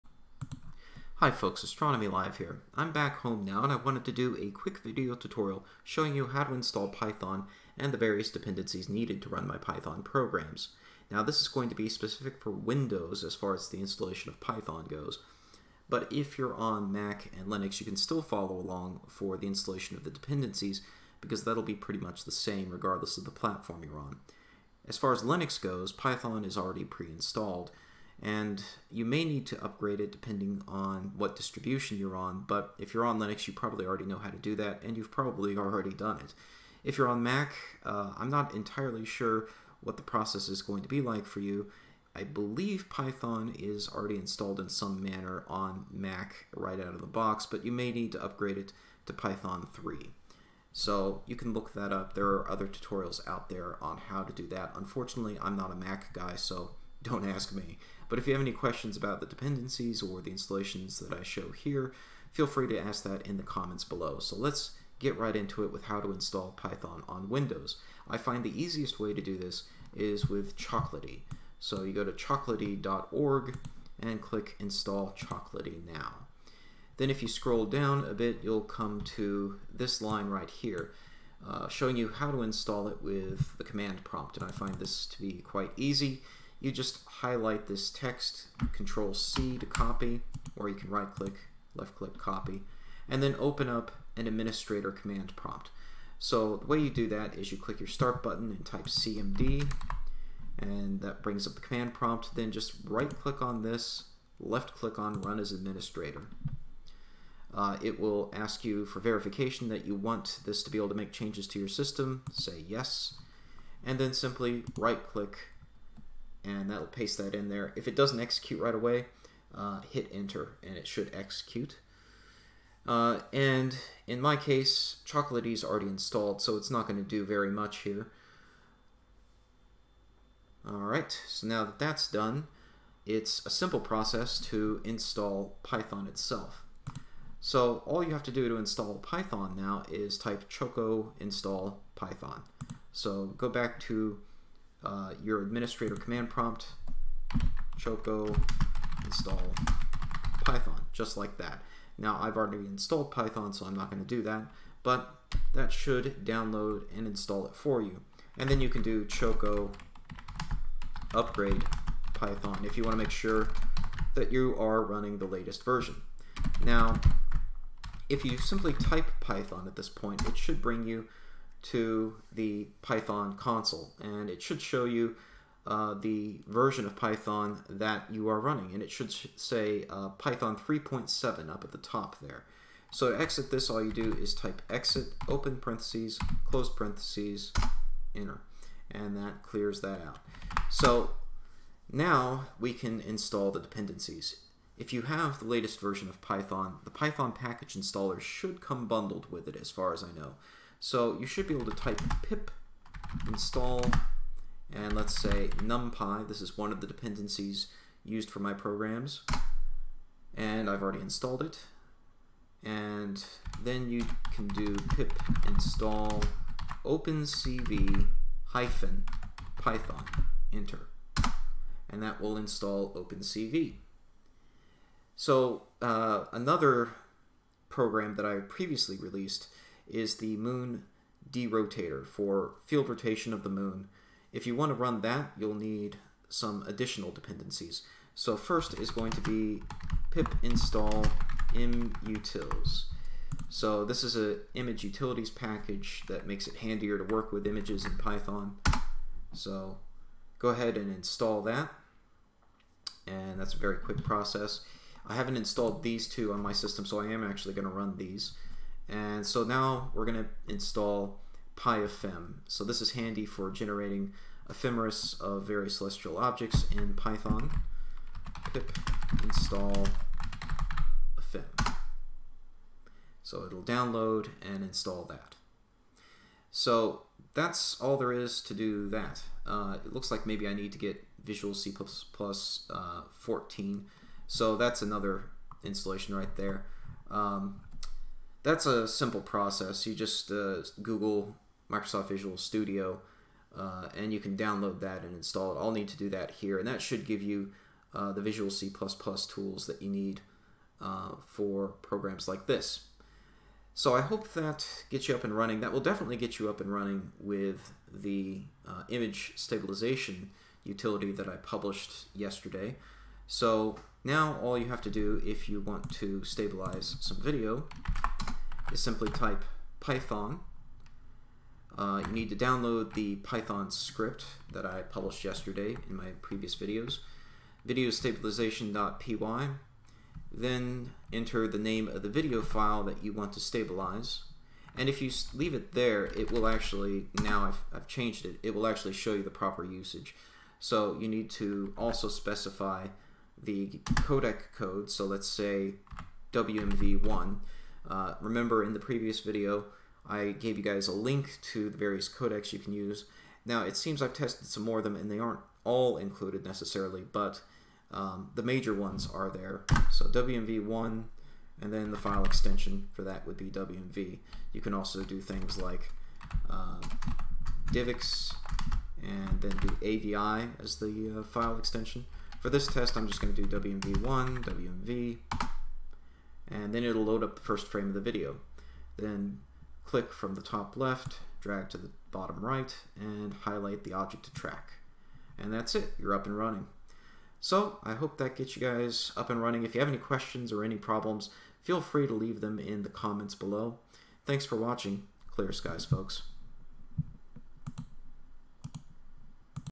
Python and OpenCV Installation Tutorial
Quick tutorial on how to install python and the dependencies used for my video stabilization software: